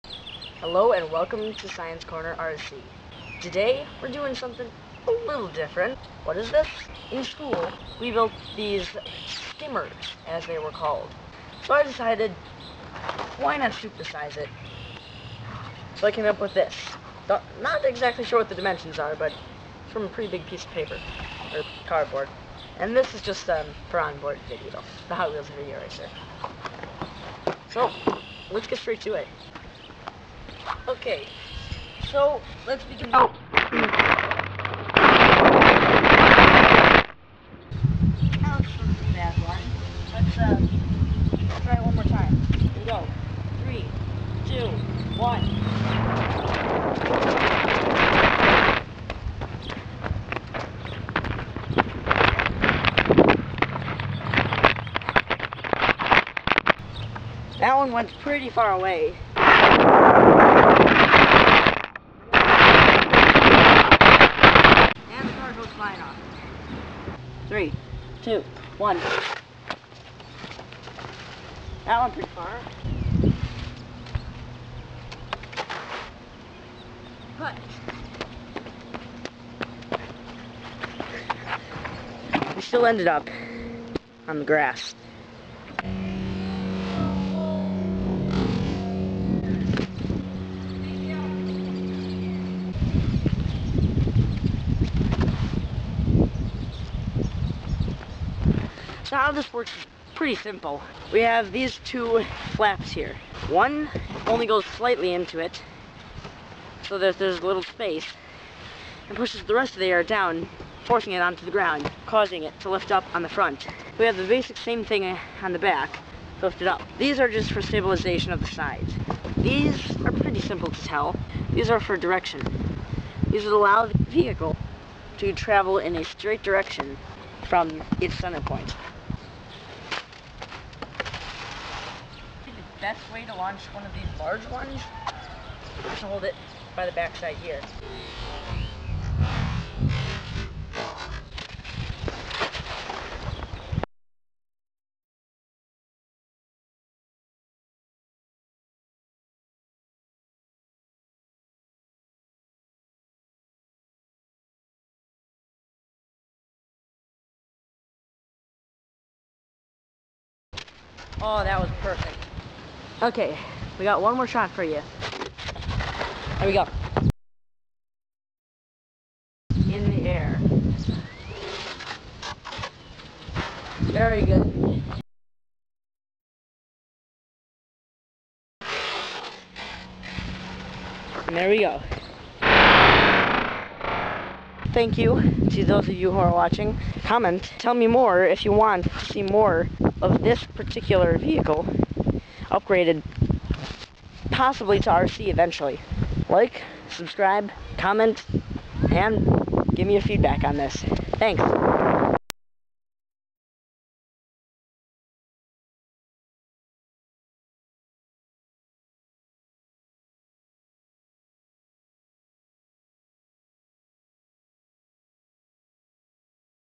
The Massive PLTWSkimmer
After building the PLTW skimmer in school, I decide to super size it!